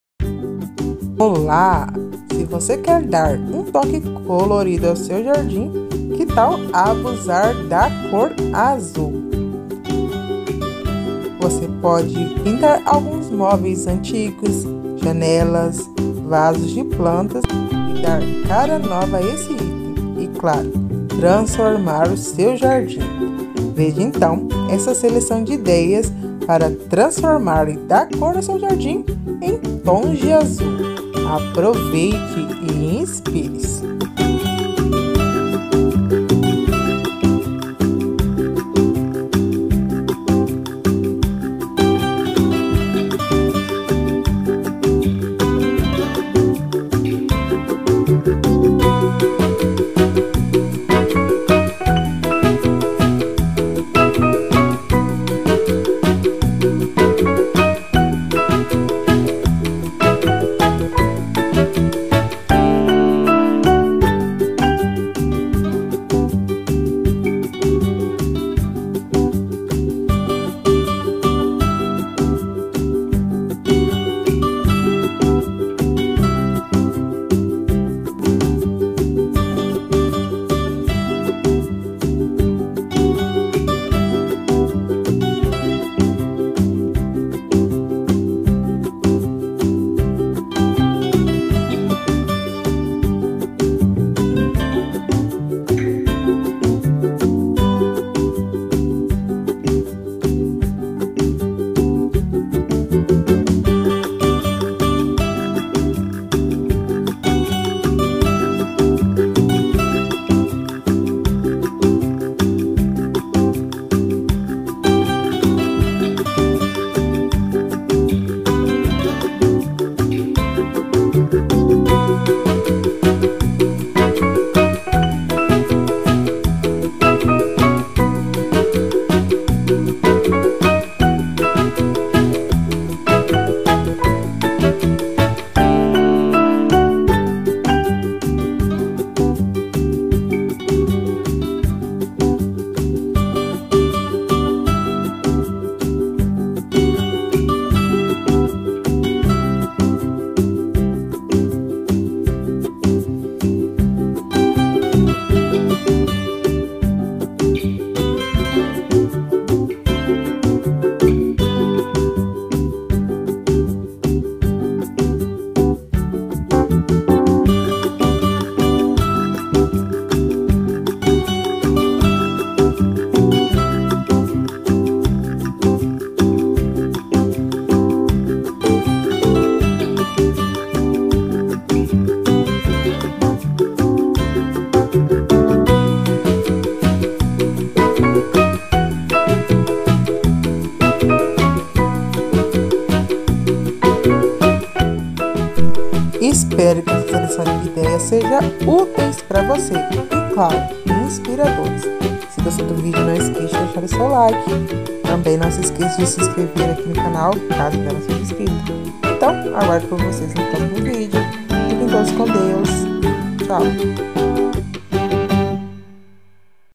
Decoração azul para ambientes externos! ideias para replicar e colorir!
Se você gosta de uma decoração criativa, colorida e bem original, veja só essa seleção de ideias de pura inspiração que você vai amar!